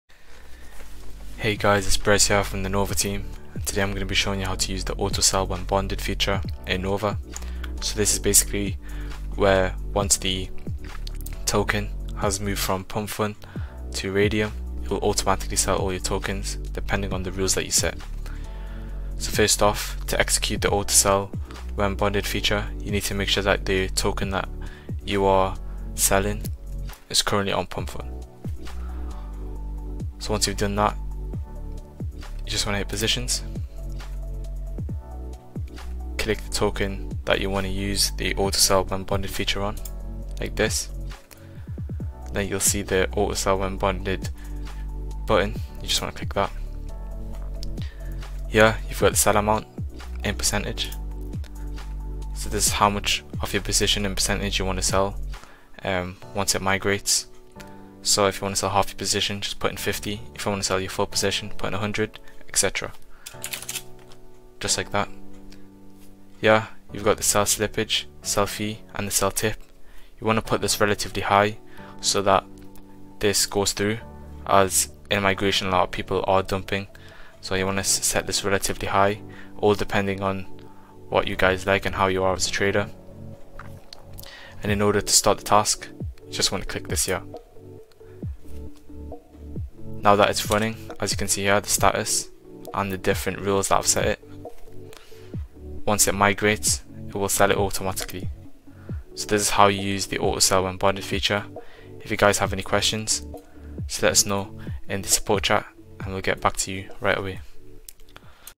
Auto-Sell when Bonding Tutorial
Here is a guide on how to auto-sell tokens at bonding using Nova.